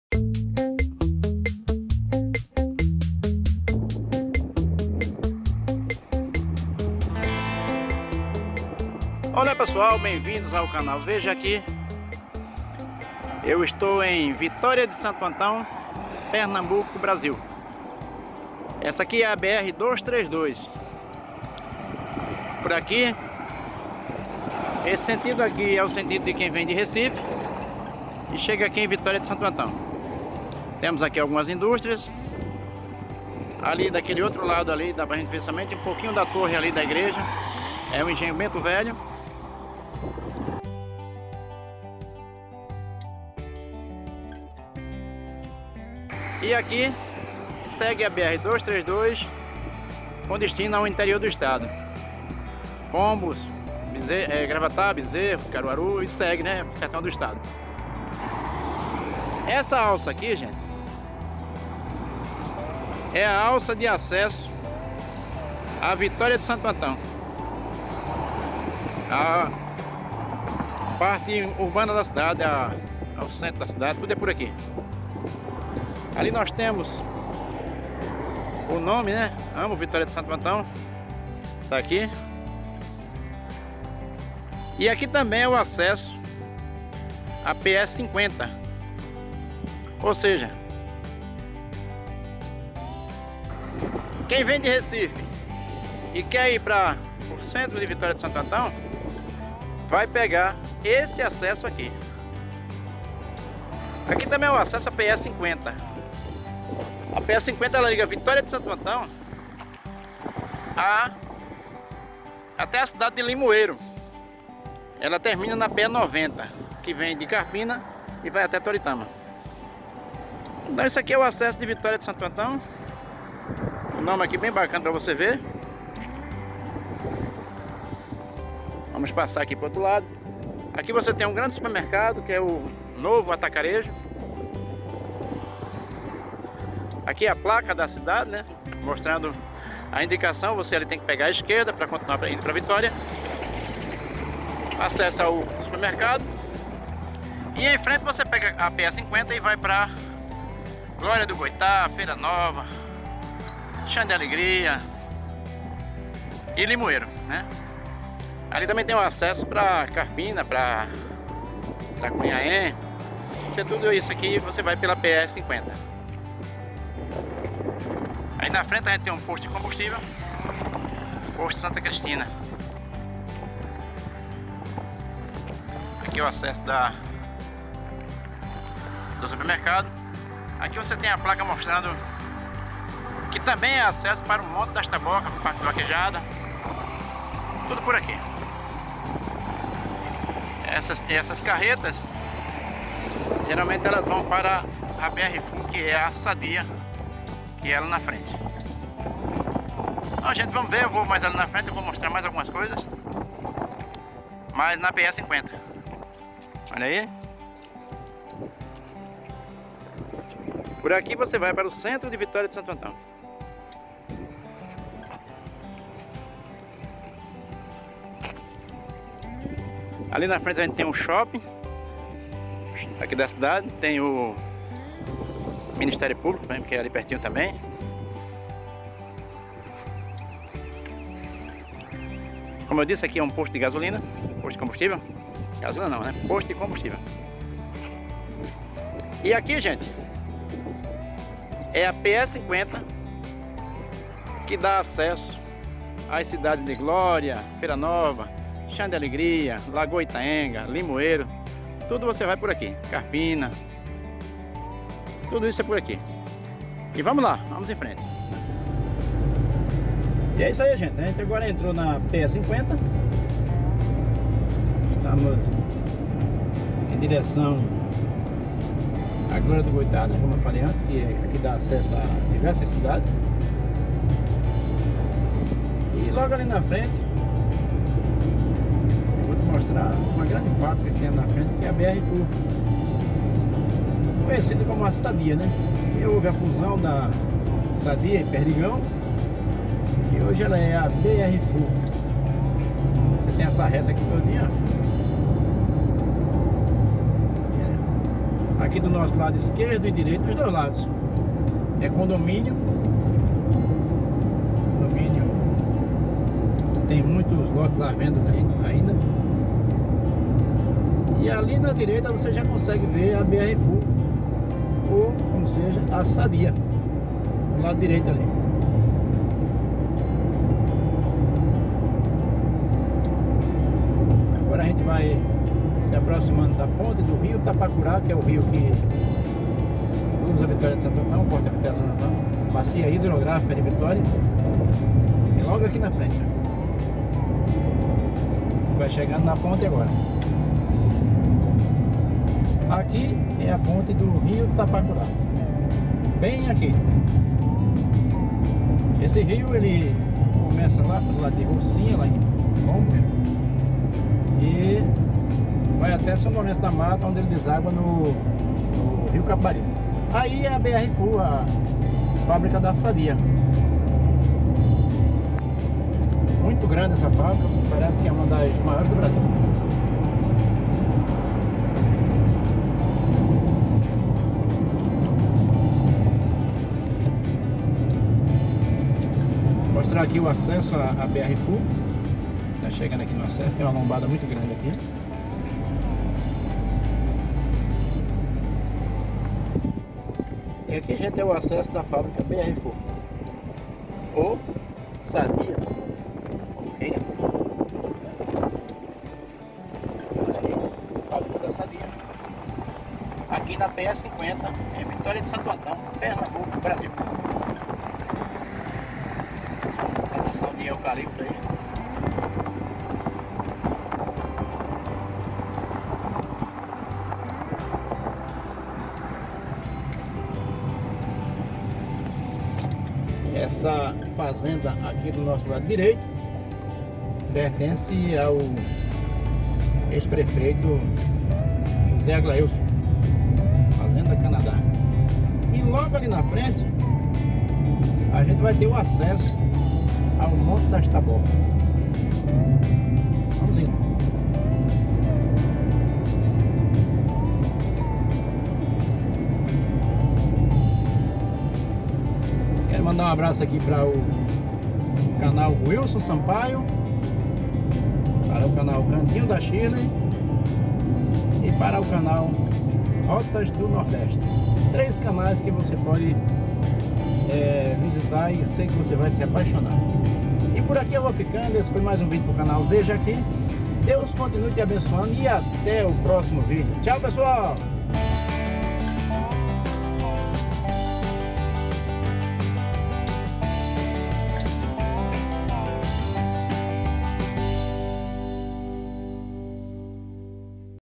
Como chegar em Vitória de Santo Antão - PE
Olá pessoal, sou João Raimundo e neste vídeo estou mostrando o acesso à cidade de Vitória de Santo Antão e também pra outras cidade da Zona da Mata e Agreste de Pernambuco.